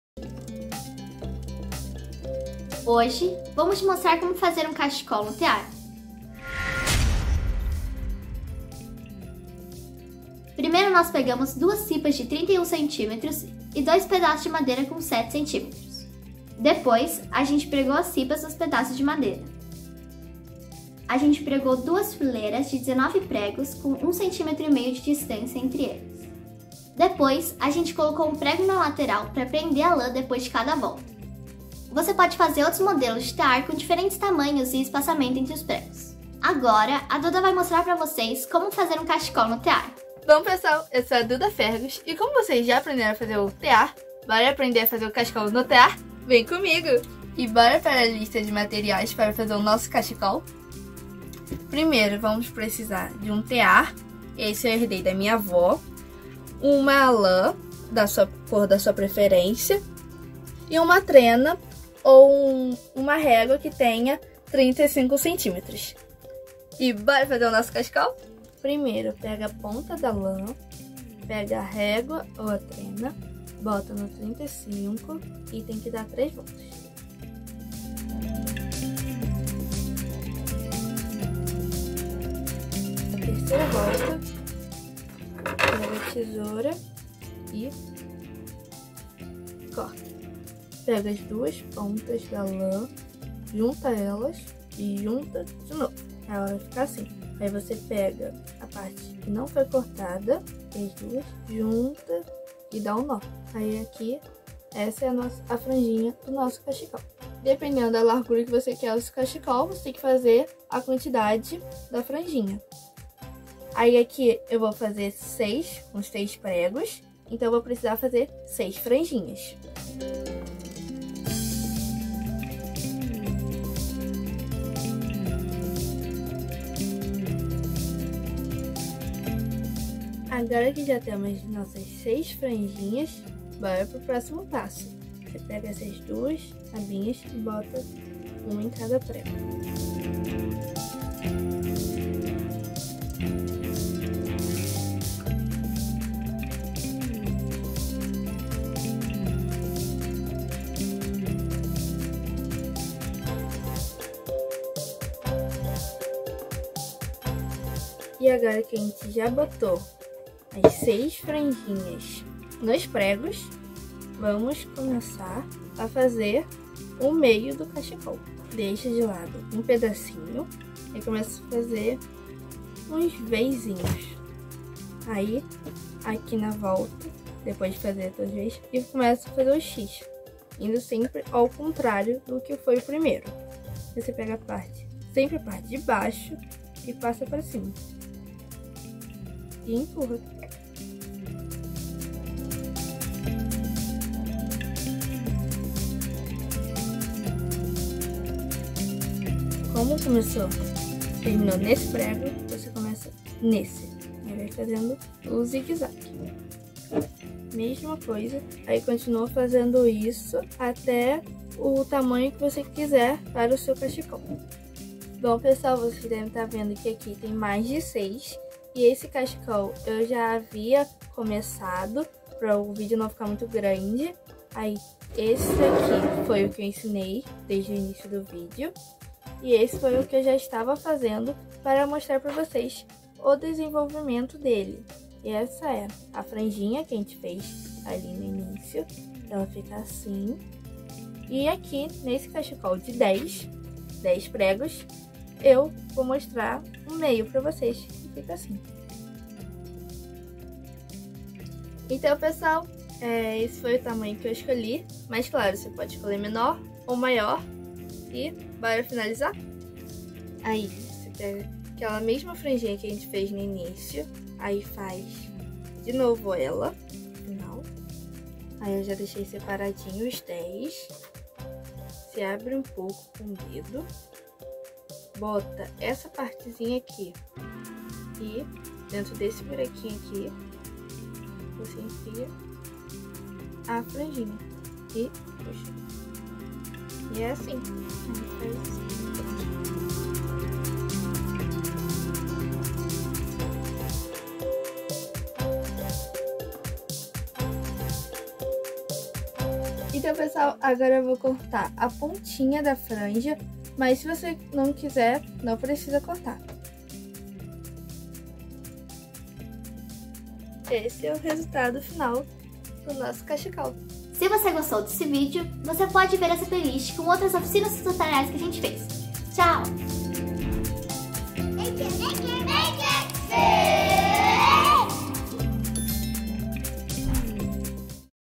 Como fazer um cachecol no tear de pregos
Veja este vídeo no qual ensinamos como fazer um tear de pregos e demonstramos como criar um cachecol utilizando-o.

Evelyn Koba(apresentação e edição), Duda Fergus, (apresentação), Marcio Koba (coordenação)